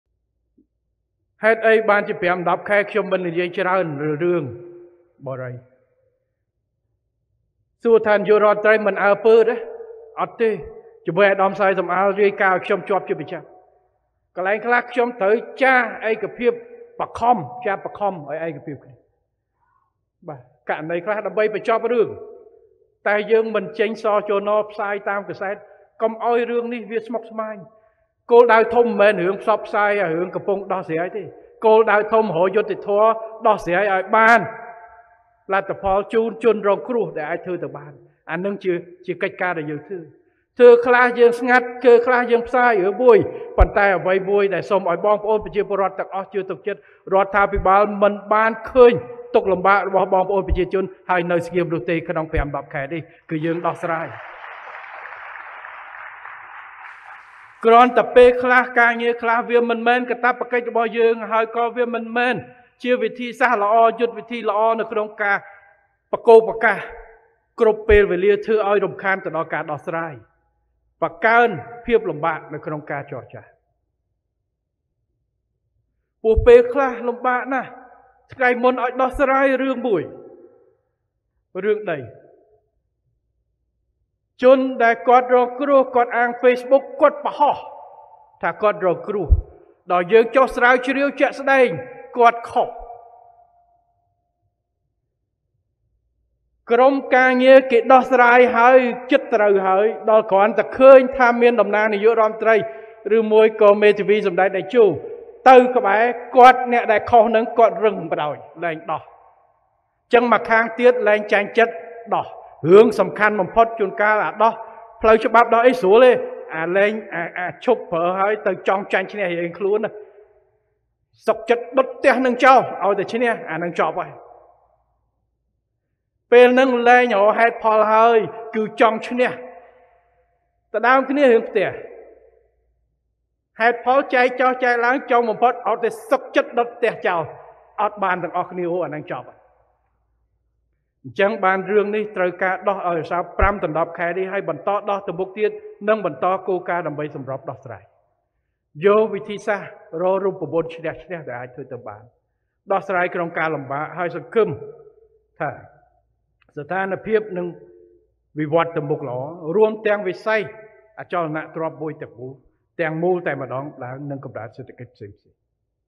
(ប្រសាសន៍ដើម)៖ សម្តេចមហាបវរធិបតី ហ៊ុន ម៉ាណែត៖ ១៥ខែកន្លងមកនេះ រាជរដ្ឋាភិបាលមិនបានដេកមើលទុក្ខលំបាក...
សម្តេចមហាបវរធិបតី ហ៊ុន ម៉ាណែត៖ ១៥ខែកន្លងមកនេះ រាជរដ្ឋាភិបាលមិនបានដេកមើលទុក្ខលំបាក របស់ប្រជាពលរដ្ឋរងគ្រោះ ចាញ់បោកក្រុមវិនិយោគបានការប្រាក់ខ្ពស់នោះទេ គ្រាន់តែរឿងខ្លះចាត់វិធានការ តែមិនផ្សព្វផ្សាយប៉ុណ្ណោះ

(ភ្នំពេញ)៖ សម្តេចមហាបវរធិបតី ហ៊ុន ម៉ាណែត នាយករដ្ឋមន្រ្តីកម្ពុជា បានថ្លែងឲ្យដឹងថា ក្នុងរយៈពេល១៥ខែកន្លងនេះ រាជរដ្ឋាភិបាលកម្ពុជា មិនបានដេកមើលទុក្ខលំបាក របស់ប្រជាពលរដ្ឋរងគ្រោះ ចាញ់បោកក្រុមវិនិយោគបានការប្រាក់ខ្ពស់នោះទេ គឺមានវិធានការជាហូរហែរ ប៉ុន្ដែគ្រាន់តែរឿងខ្លះ ចាប់វិធានការតែមិនធ្វើការផ្សព្វផ្សាយប៉ុណ្ណោះ។

ការលើកឡើងរបស់សម្តេចមហាបវរធិបតី ហ៊ុន ម៉ាណែត ធ្វើឡើងក្នុងឱកាសសម្ដេច អញ្ជើញបិទសន្និបាតបូកសរុប លទ្ធផលការងារឆ្នាំ២០២៣និងឆ្នាំ២០២៤ និងទិសដៅការងារឆ្នាំ២០២៥ របស់ក្រសួងរៀបចំដែនដី នគរូបនីយកម្ម និងសំណង់ នារសៀលថ្ងៃទី២៦ ខែវិច្ឆិកា ឆ្នាំ២០២៤។

សម្តេចមហាបវរធិបតីបានថ្លែងថា «ហេតុអ្វី១៥ខែមកនេះ ខ្ញុំមិនបាននិយាយច្រើនអំពីរឿងបុរី បើសួរត្រឡប់ថា នាយករដ្ឋមន្រ្ដីមិនអើពើឬយ៉ាងម៉េច? គឺមិនមែនទេ ឯកឧត្តម សាយ សំអាល បានរាយការណ៍ដល់ខ្ញុំជាប់ជាប្រចាំ កន្លែងខ្លះខ្ញុំត្រូវចារណ៍ឯកភាពបង្ខំទៅលើករណីខ្លះ ដើម្បីបញ្ចប់រឿង តែគ្រាន់តែយើងមិនចេញ ស.ជ.ណ ផ្សព្វផ្សាយតាមសារព័ត៌មាន ដើម្បីកុំឲ្យរឿងនេះស្ម័គ្រស្មាញ គោលដៅធំមិនមែនផ្សព្វផ្សាយរឿងកំពុងដោះស្រាយនោះទេ គោលដៅធំ គឺត្រូវរកយុត្តិធម៌ដោះស្រាយឲ្យបានលទ្ធផល ជូនជនរងគ្រោះឲ្យបាន»។

សម្តេចមហាបវរធិបតីបានបញ្ជាក់ថា រឿងខ្លះយើងផ្សព្វផ្សាយ រឿងខ្លះក៏ស្ងាត់ ប៉ុន្ដែអ្វីដែលធ្វើឲ្យប្រជាពលរដ្ឋជឿទុកចិត្ត គឺថា រាជរដ្ឋាភិបាលមិនបានឃើញទុក្ខលំបាក របស់បងប្អូនប្រជាជនហើយនៅស្ងាត់នោះទេ រយៈពេល១៥ខែកន្លងមកនេះ យើងបានដោះស្រាយជាហូរហែរ។ គ្រាន់តែថា កិច្ចការងារខ្លះមិនមែនកតព្វកិច្ចរបស់យើង និងមិនមែនជាវិធីសាស្រ្ដល្អ ក្នុងការប្រកូកប្រកាសគ្រប់ពេលវេលា ធ្វើឲ្យរំខាន់ដល់ការដោះស្រាយ បង្កើនភាពលំបាកក្នុងការចរចារនោះទេ ព្រោះពេលខ្លះលំបាកណាស់ ក្នុងការដោះស្រាយរឿងមួយៗ។

សម្តេចមហាបវរធិបតីបានបន្ថែមថា ជាក់ស្ដែងថ្ងៃមុនយើងបានឲ្យដោះស្រាយរឿងដីមួយកន្លែង ជនរងគ្រោះអាង Facebook បានបង្ហោះថា ខ្លួនរងគ្រោះលុះដល់ពេលចុះស្រាវជ្រាវជាក់ស្ដែង ជនរងគ្រោះនោះអ្នកខុស ក្រុមការងារបានព្យាយាមដោះស្រាយជិតត្រូវហើយ ក្រោយមកគ្រាន់តែឃើញតំណាងនាយករដ្ឋមន្ដ្រី ឬមេធាវីសម្ដេចតេជោទៅក្បែរ អ្នកដែលខុសនេះគឺរឹងទទឹងតែម្ដង ពុំព្រមដោះស្រាយនោះទេ គឺចង់ឈ្នះ[. ]នេះជាការលំបាកមួយ។ ១៥ខែកន្លងមកនេះយើងបានដោះស្រាយរឿងជាច្រើន និងបន្ដដោះស្រាយទៅមុខទៀត និងបន្ដគោលការណ៍ដើម្បីដោះស្រាយ។

សម្តេចមហាបវរធិបតី ហ៊ុន ម៉ាណែត បានថ្លែងបន្ថែមថា យើងបានរករូបមន្ដ វិធីសាស្រ្ដឈ្នះ ឈ្នះ ដែលអាចធ្វើទៅបាន ដោះស្រាយក្នុងការលំបាក និងសង្ឃឹមថា ស្ថានភាព និងវិត្តន៍ទៅមុខល្អ រួមទាំងវិស័យអចលនទ្រព្យទាំងមូល៕